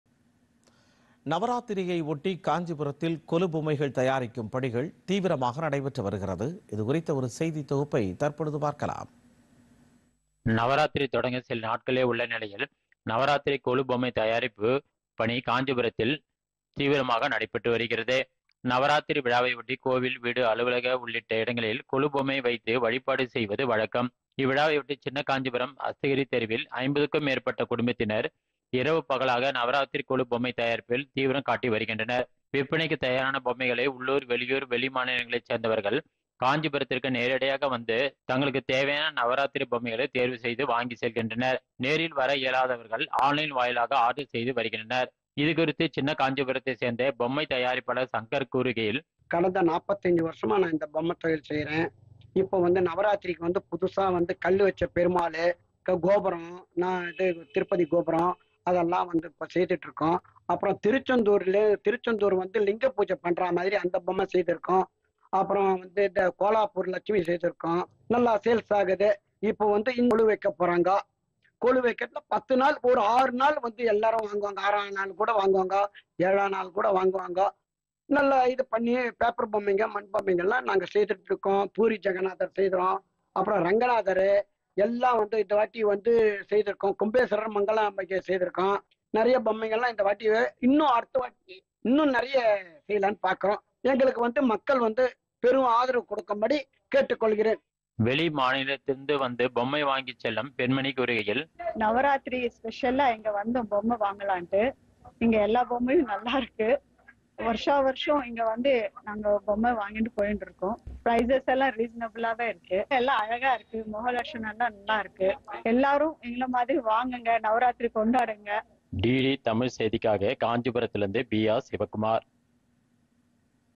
காஞ்சிபுரம்: கொலு பொம்மைகள் தயாரிக்கும் பணிகள் தீவிரம்
நவராத்திரியை ஒட்டி காஞ்சிபுரத்தில் கொலு பொம்மைகள் தயாரிக்கும் பணிகள் தீவிரமாக நடைபெற்று வருகிறது.

இது குறித்த ஒரு செய்தி தொகுப்பு..